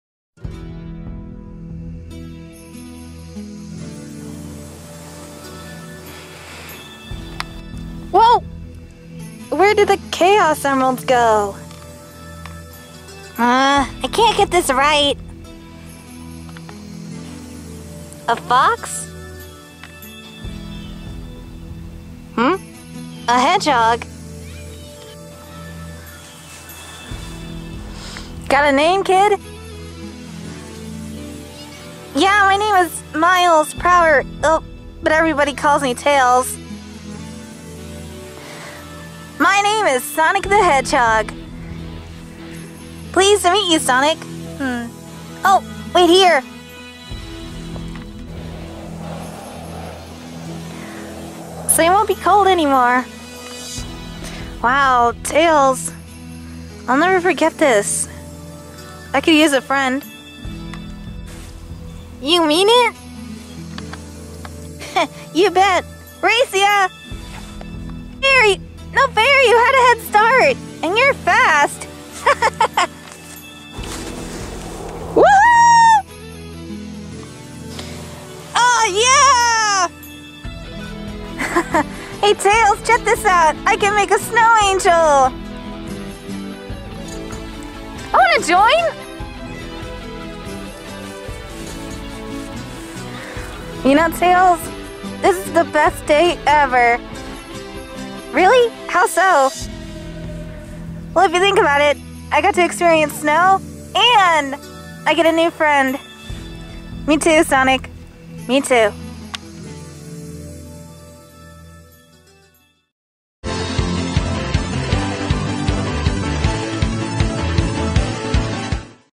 Sonic Plush X Short: Sonic & Tails' Origins
Ever wondered how did Sonic and Tails meet in my series? Here it is!
And yes, I took advantage of a snow day.